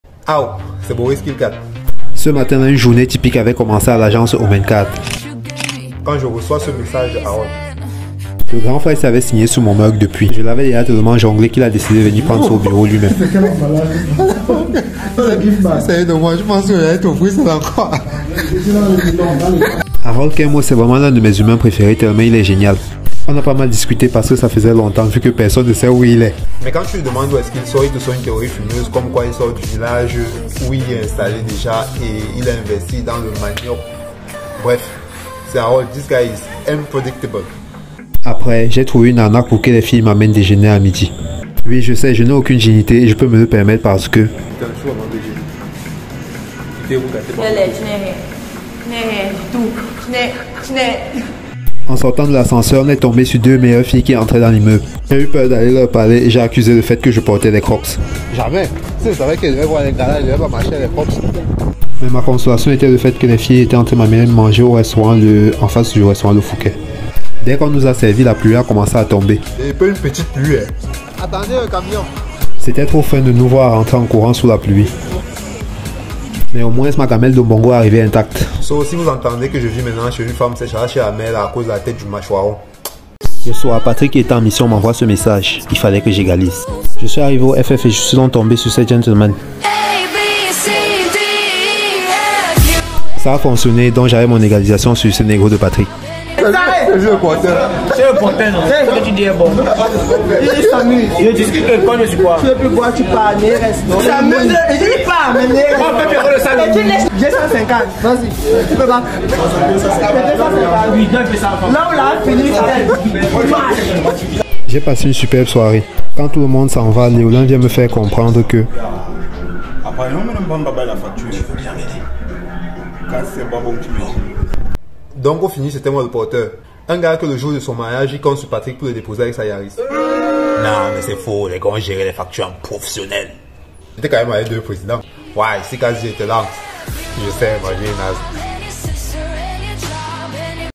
Un nouveau vlog sur ma vie à Douala
Harold vient piquer mon mug... je me fais payer à bouffer... bref un nouveau vlog sur moi à Douala eeeeeeh